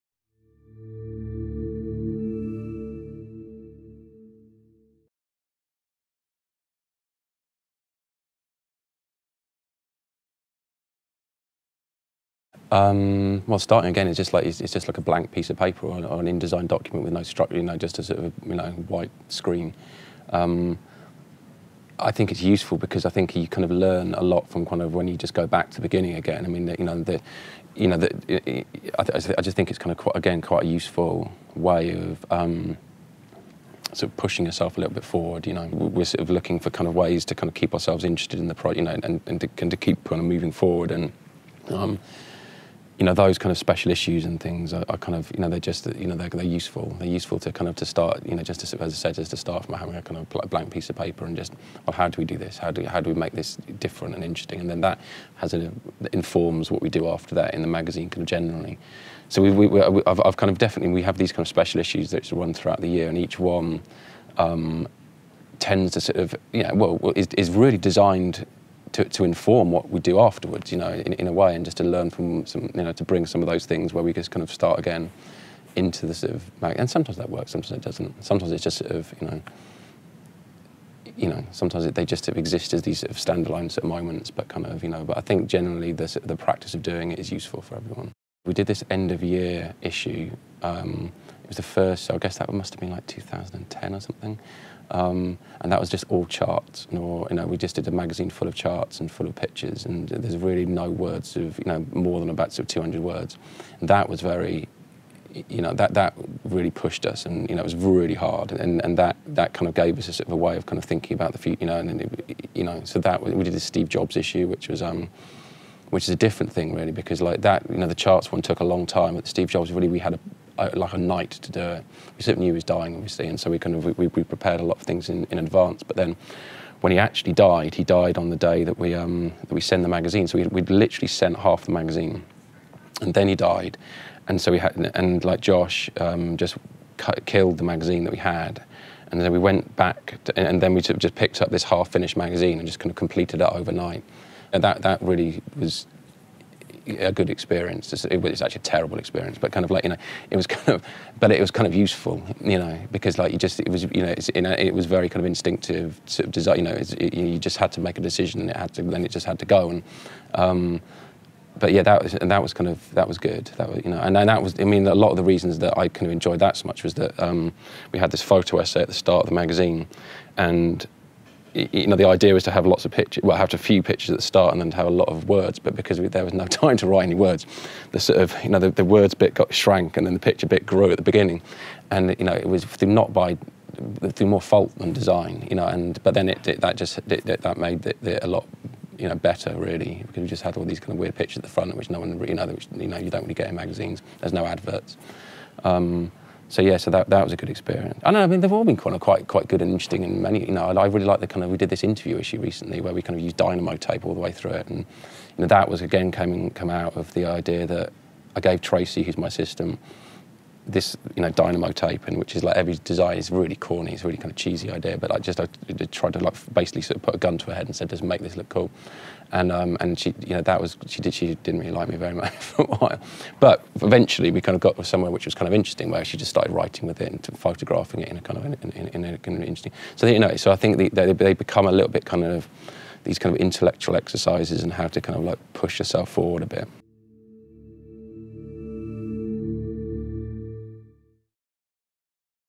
The Designer Series: Richard Turley (part 5)
The power of dynamo tape, and why starting again is the best way to push yourself forward